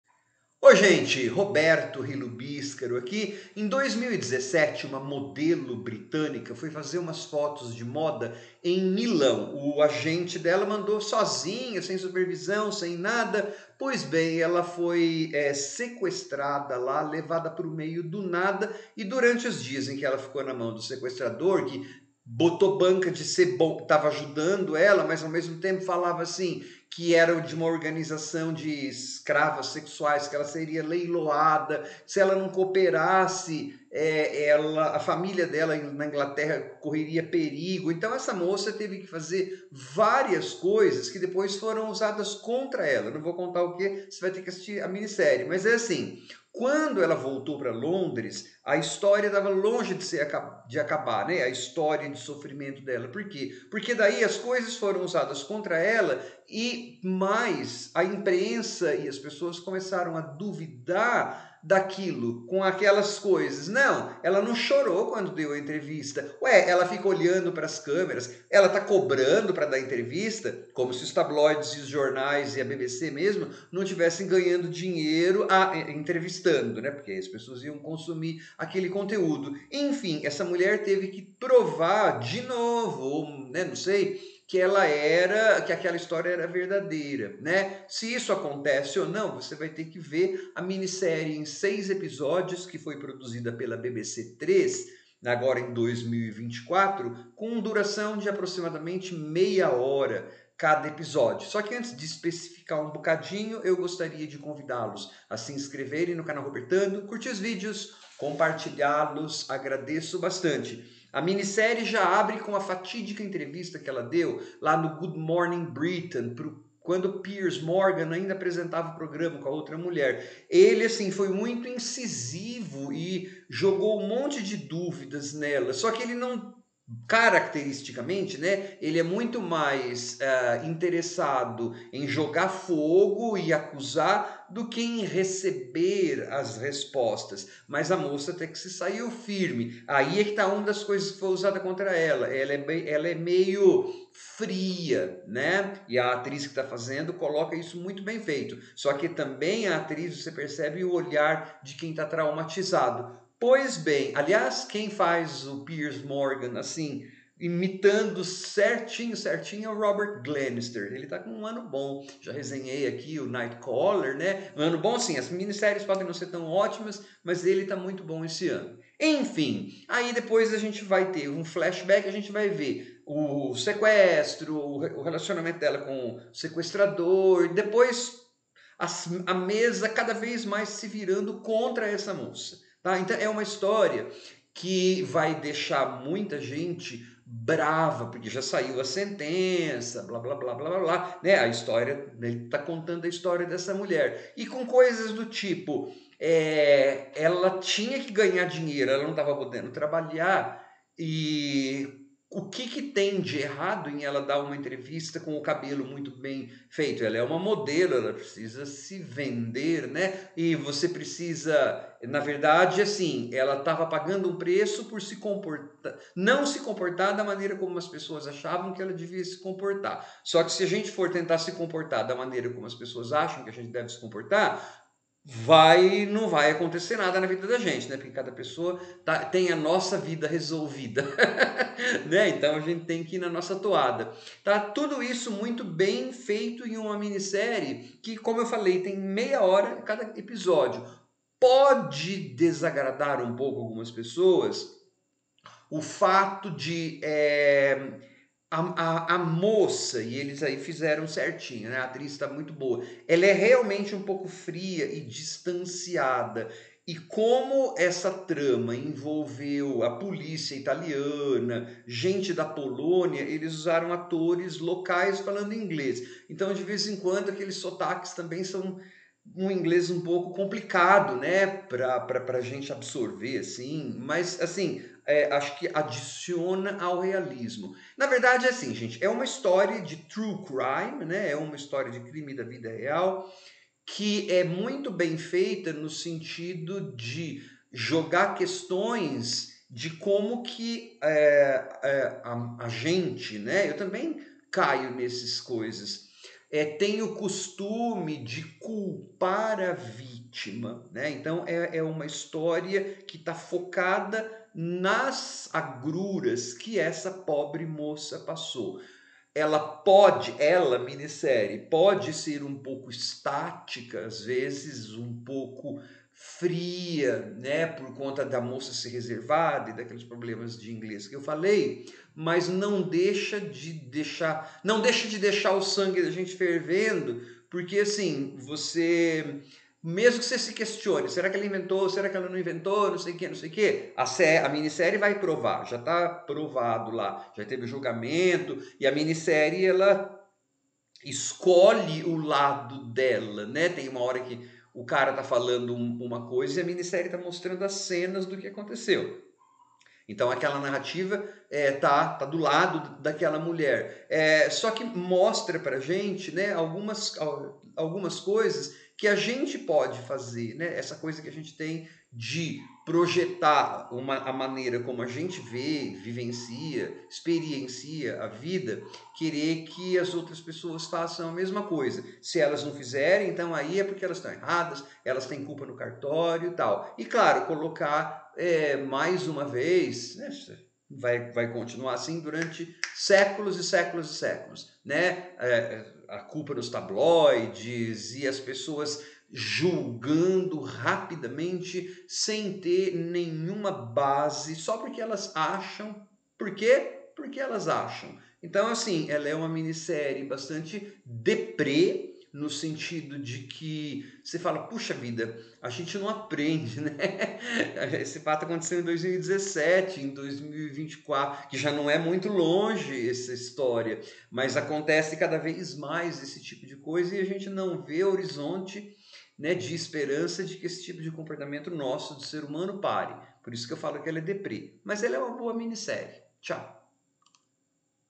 Crítica da minissérie Kidnapped: The Chloe Ayling Story (2024)
Acompanha uma modelo glamourosa, sequestrada enquanto trabalha na Itália.